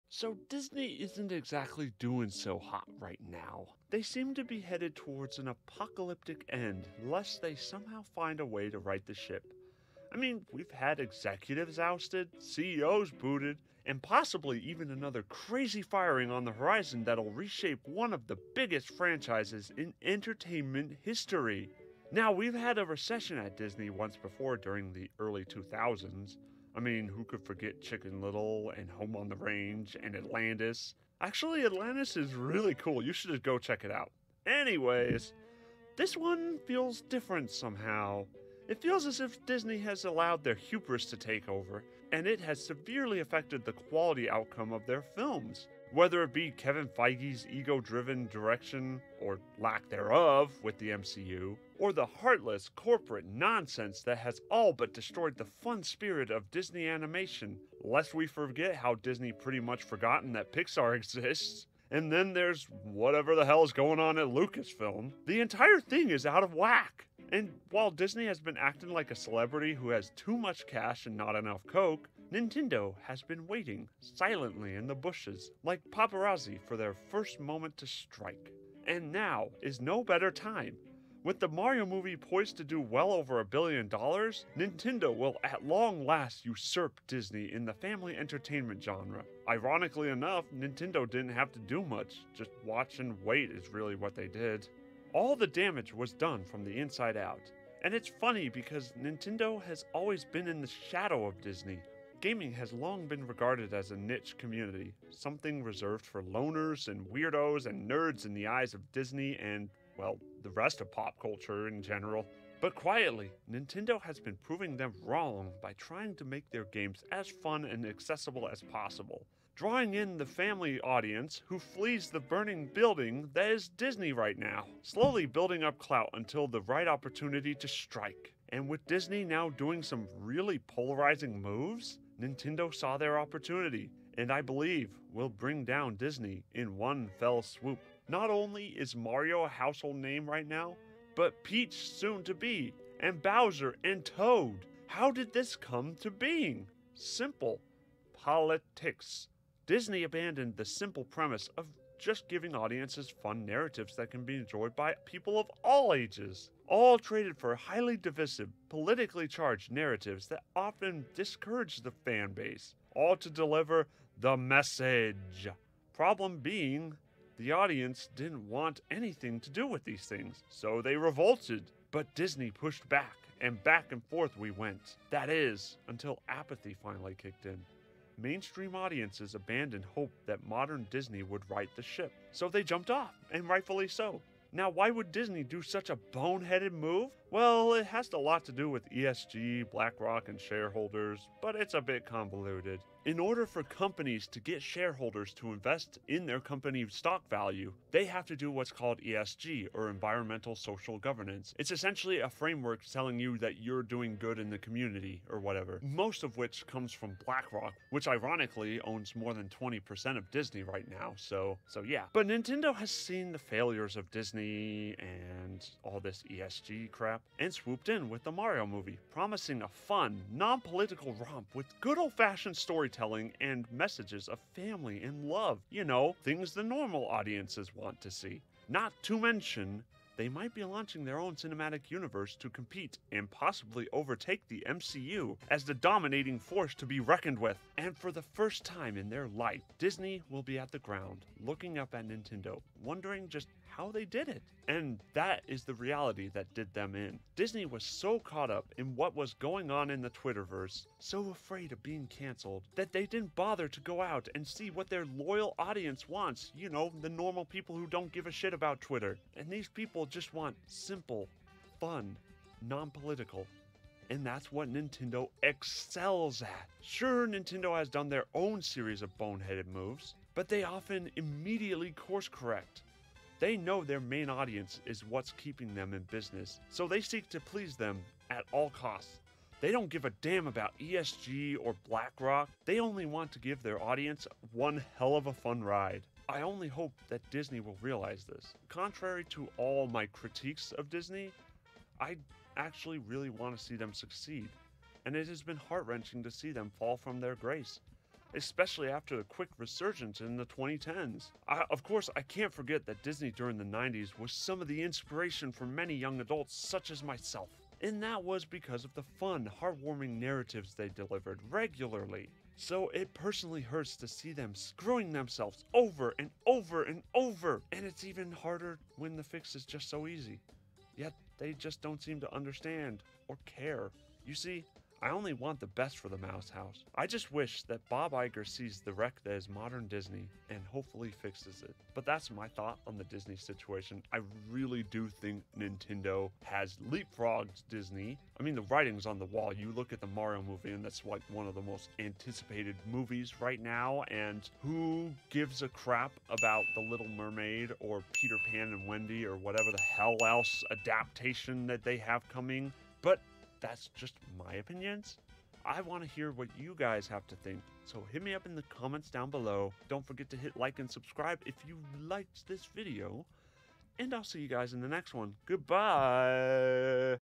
Nintendo is Eating Disney's Lunch
Disney is in a freefall...

Disney has been hemorrhaging executives and employees. Many of their projects, including the once reliable Marvel and Star Wars, have been failing to produce results. And that's because Disney has put more focus on divisive politics rather than fun family-friendly products.

And that's where Nintendo steps in.

Nintendo has been doing what Disney should... giving fun stories while staying apolitical. With the Mario Movie poised to do a BILLION dollars at the box office, Nintendo will seal Disney's fate.

This is my assessment of the situation.